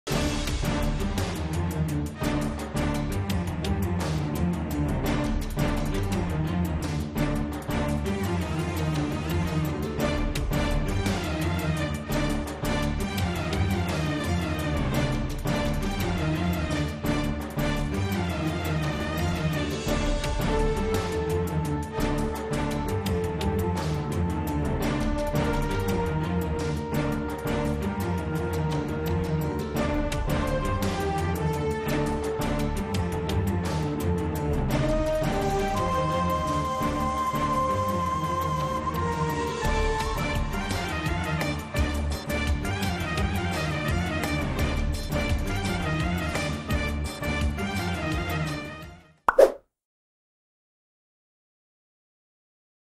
Three-person tightrope walking| CCTV English
Three tightrope walkers show off their balancing skills on the high wire with smooth cooperation.